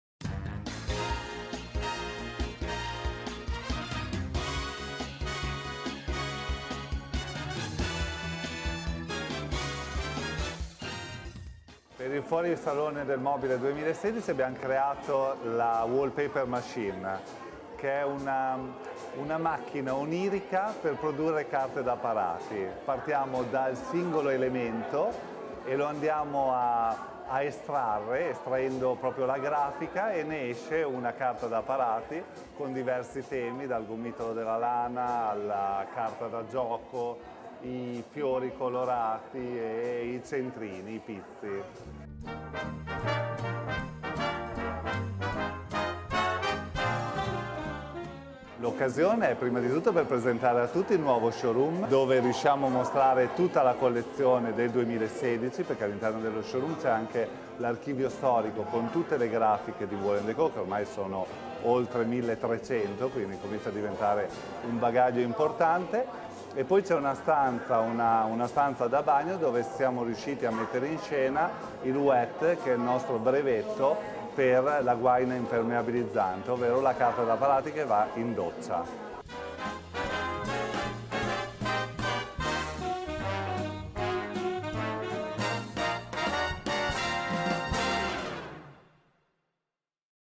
Wall&decò @ Fuorisalone 2016
Il nuovo showroom e la wallpaper machine al Furoisalone 2016 / The new showroom and wallpaper machine at Fuorisalone 2016